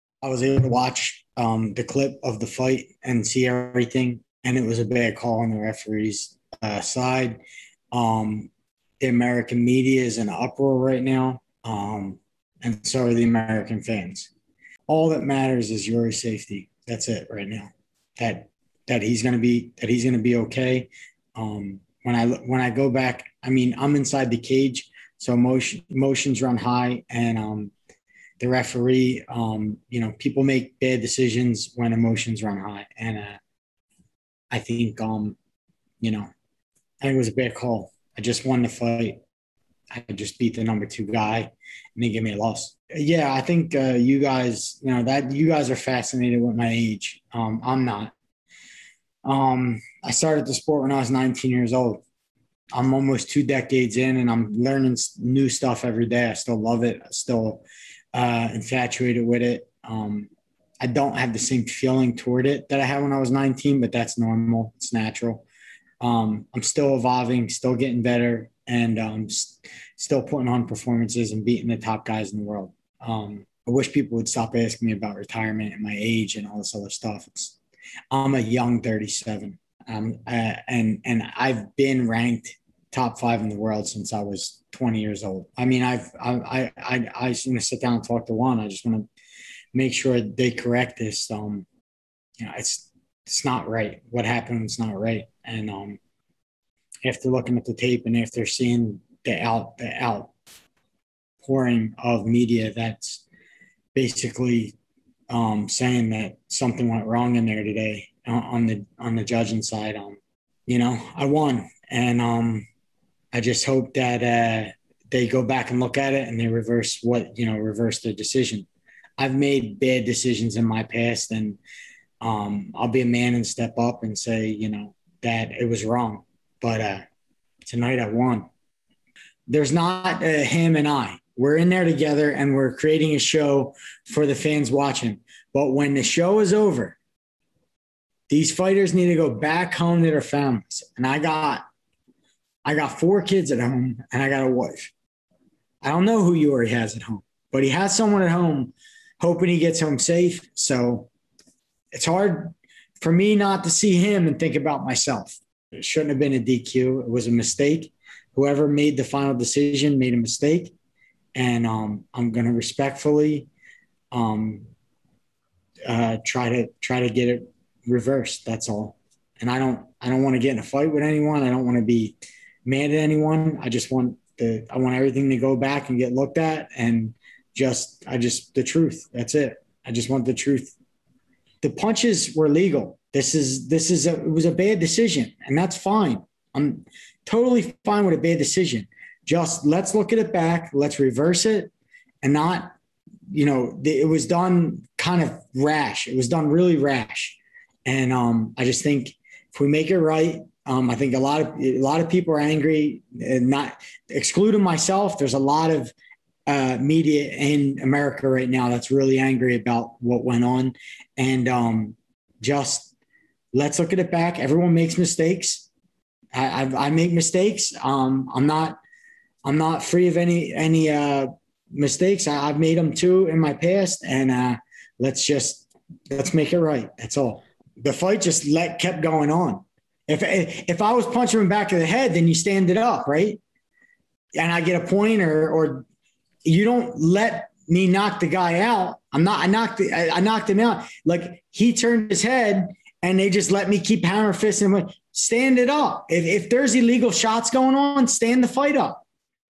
Eddie Alvarez questions DQ loss for illegal punches | ONE on TNT I
Former UFC/Bellator champ Eddie Alvarez spoke to the media following his disqualification loss at ONE on TNT I due to illegal punches to the back of Iuri Lupicas' head.